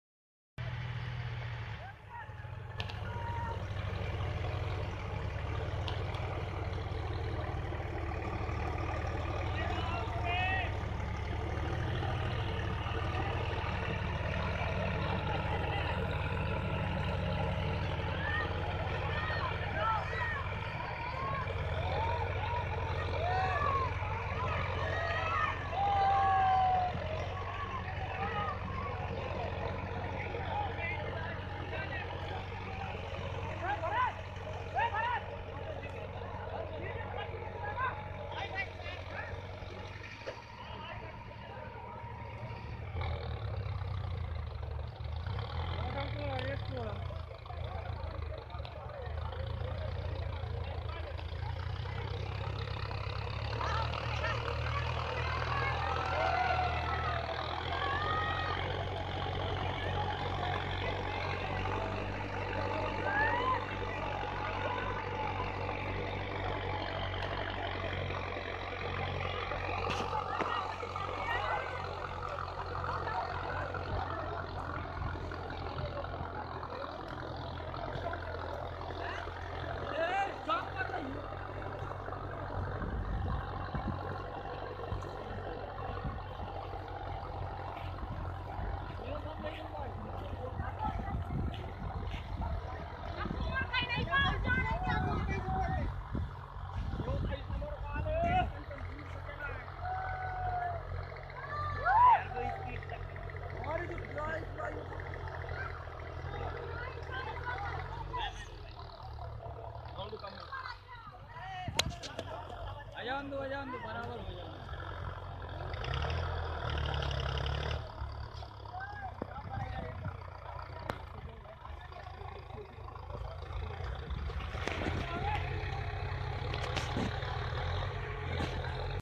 Deutz Fahr Palti Plow vs Mahindra Arjun Palti Plow*
*मुख्य विशेषताएं:*

*Deutz Fahr Palti Plow:*

1. इस ट्रैकर की शक्तिशाली पलटने की क्षमता है
2. उच्च गुणवत्ता चलने वाले टायर
3. आसानी से संचालित करने योग्य
4. उच्च गति पर भी स्थिरता
5. विभिन्न प्रकार के खेतों में उपयोग करने योग्य

*Mahindra Arjun Palti Plow:*

1. उच्च प्रदर्शन चलने वाले टायर
2. कम ईंधन खपत
3. आसानी से रख-रखाव करने योग्य
4. विभिन्न प्रकार के फसलों के लिए उपयुक्त
5. उच्च गुणवत्ता वाले निर्माण

*तुलना:*

1. *पलटने की क्षमता:* Deutz Fahr पल्टी प्लाऊ को महिंद्रा अर्जुन पल्टी प्लाऊ की तुलना में अधिक शक्तिशाली है।
2. *ईंधन खपत:* महिंद्रा अर्जुन पल्टी प्लाऊ को ड्यूट्ज फहर पल्टी प्लाऊ की तुलना में कम ईंधन खपत करता है।
3. *रख-रखाव:* महिंद्रा अर्जुन पल्टी प्लाऊ को रख-रखाव ड्यूट्ज फहर पल्टी प्लाऊ की तुलना में आसान है।
4. *गुणवत्ता:* दोनों ट्रेक्टर पल्टी प्लाऊ उच्च गुणवत्ता वाले हैं, लेकिन ड्यूट्ज फहर की पल्टी प्लाऊ की गुणवत्ता थोड़ी अधिक है।

*निष्कर्ष:*

दोनों पल्टी प्लाऊ को अपने आप में उत्कृष्ट हैं, लेकिन आपकी आवश्यकताओं और प्राथमिकताओं के आधार पर चुनाव करना होगा। यदि आप अधिक शक्तिशाली पलटने की क्षमता चाहते हैं, तो Deutz Fahr पल्टी प्लाऊ अच्छा विकल्प हो सकता है। यदि आप कम ईंधन खपत और आसान रख-रखाव चाहते हैं, तो Mahindra Arjun पल्टी प्लाऊ अच्छा विकल्प हो सकता है।

Places waching all videos 🙏🙏